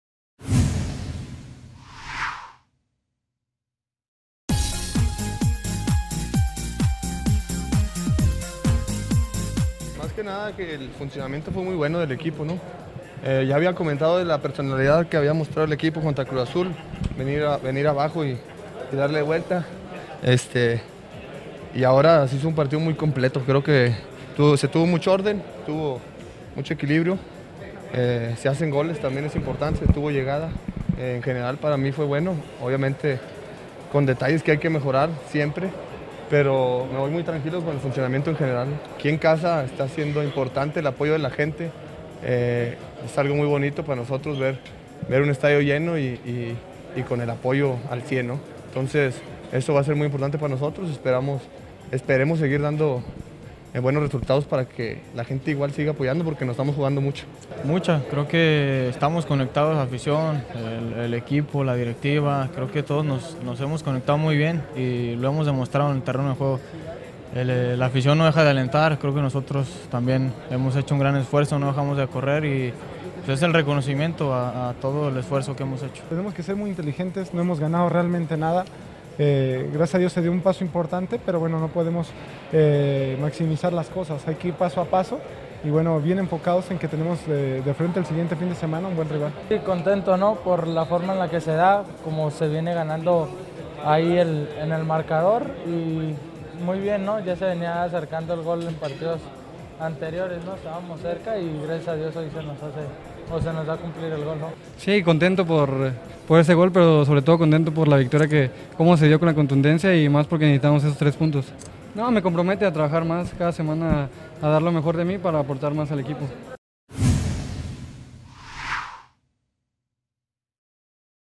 Reacciones del Chivas 3 0 Monterrey J8 CL2015
Esto comentaron nuestros jugadores tras la victoria en casa ante Monterrey por 3-0. JuntosVsTodo ¡Gracias afición!